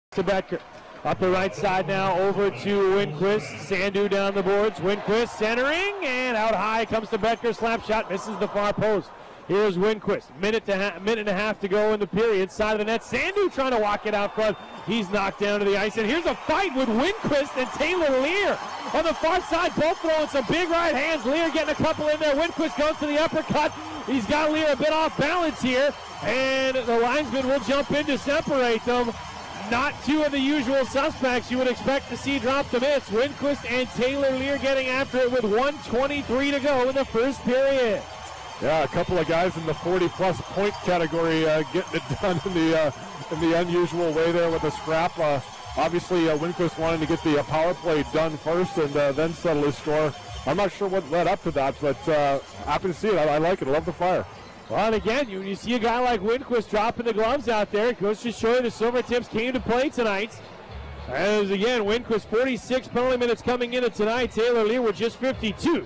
Taylor Leier vs Josh Winquist Mar 2, 2013
Taylor Leier vs Josh Winquist from the Portland Winterhawks at Everett Silvertips game on Mar 2, 2013.